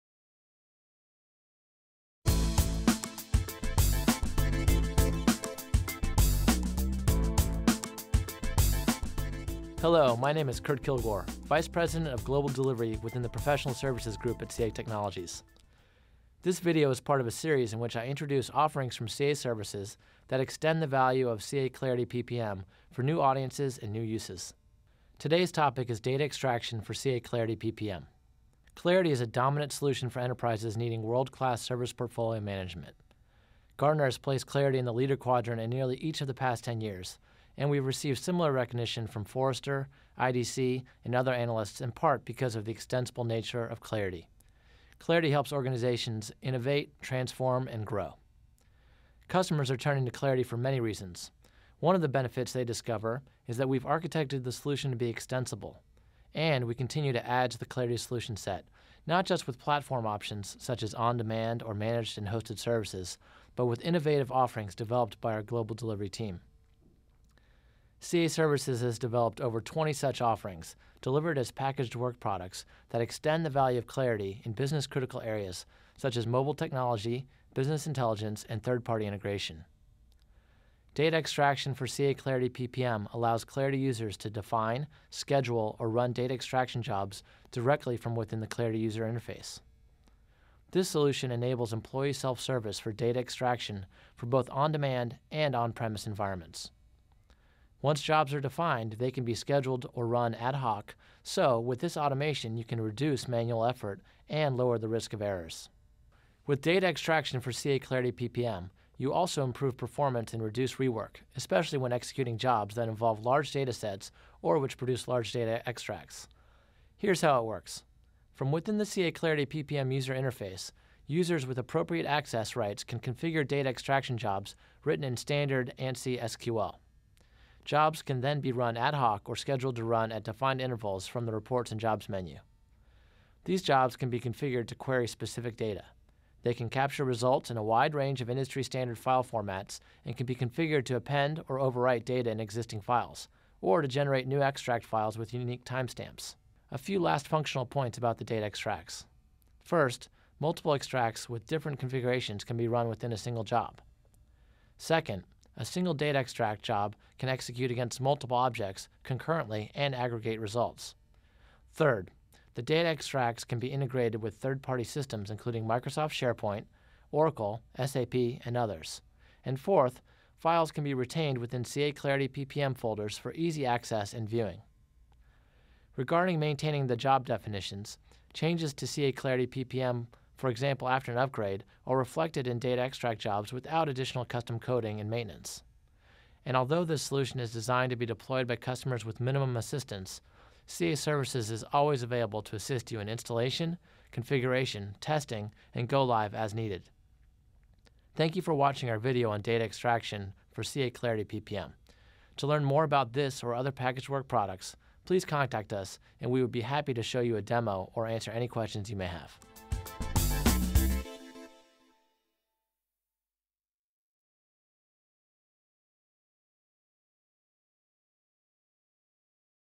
Data Extraction for CA Clarity PPM
In this video, Kurt Kilgore, VP of Global Delivery for CA Technologies, provides an overview of Data Extraction for CA Clarity PPM.

This innovative packaged work product from CA Services extends the value for CA Clarity PPM customers.

CA Clarity PPM customers can efficiently extract valuable PPM data for use in 3rd party systems such as Microsoft SharePoint, Oracle and SAP.